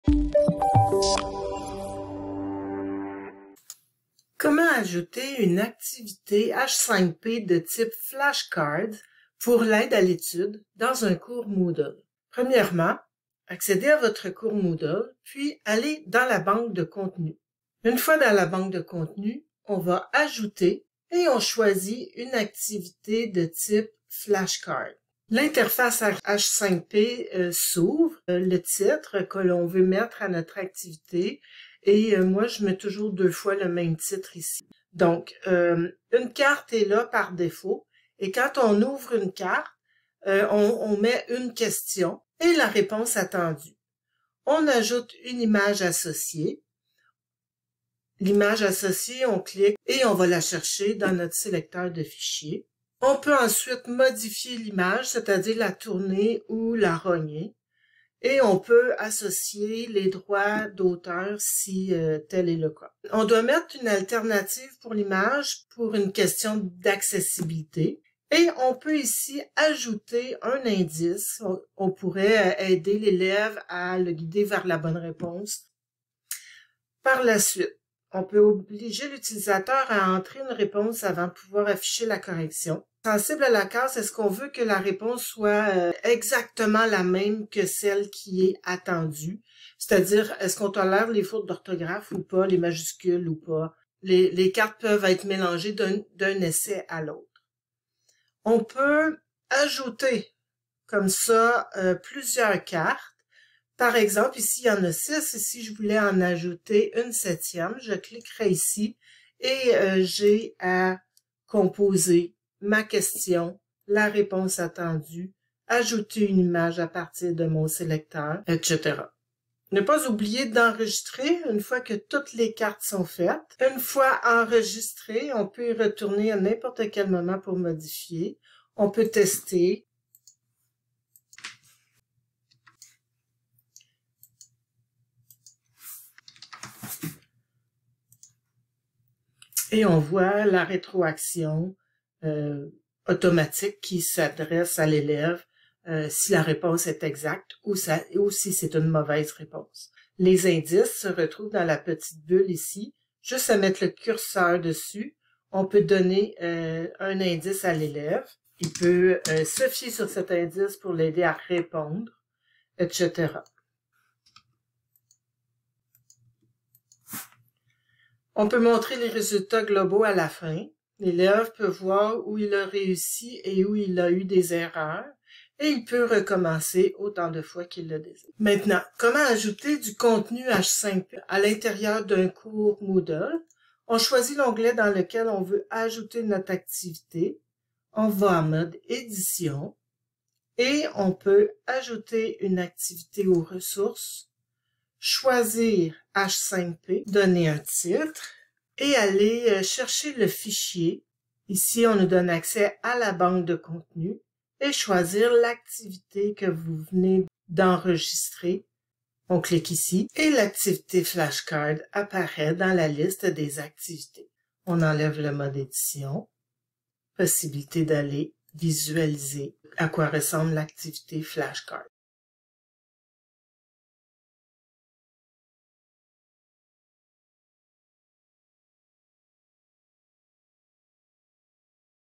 Flashcards-H5P
Comment créer des cartes d'aide è l'étude de type Flashcards de H5P dans votre cours moodle?